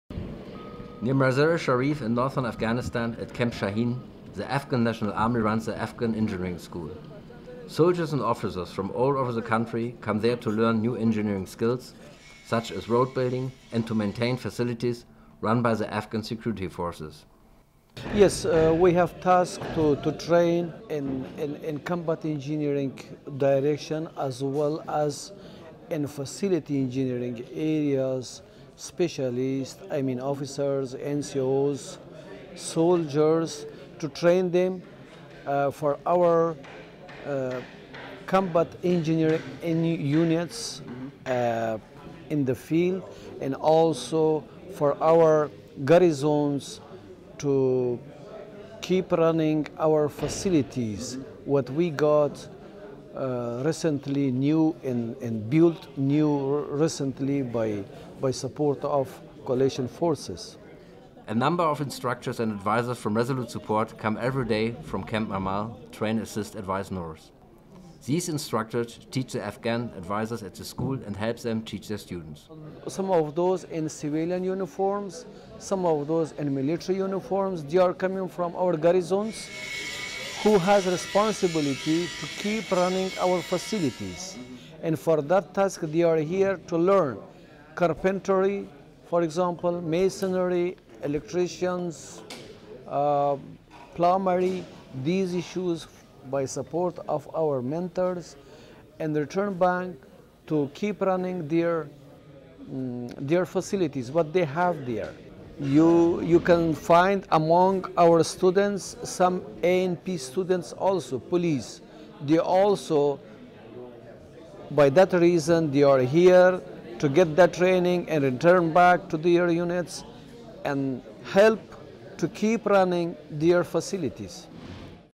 The Afghan National Army Engineering school
At Camp Shaheen, near Mazar I Sharif, the Engineering school of the Afghan National Army trains soldiers and officers to do combat engineering and maintenance of the ANA facilities all over Afghanistan. Every day, instructors from Resolute Support´s Train Advise Assist North come to the school to teach the students and assist the Afghan trainers to do their job.